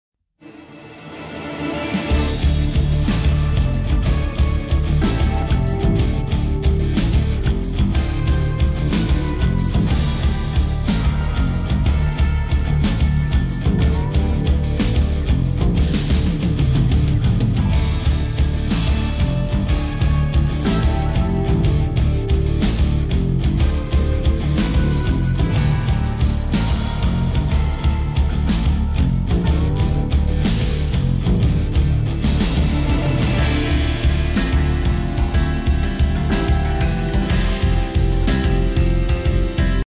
CEDESI ATTIVITA' MINIMARKET IN CENTRO
A Carate Brianza, nella centralissima via Dante Cesana, proponiamo cessione di attività di minimarket. L'attività si trova in una zona di forte passaggio, sia pedonale che in macchina, e si colloca in negozio ben tenuto con due ampi locali, più un retro e servizio e uscita su cortiletto interno (dove permesso carico e scarico), e con contratto regolare di affitto 4+4. Tre ampie vetrine su strada permettono un'ampia visibilità.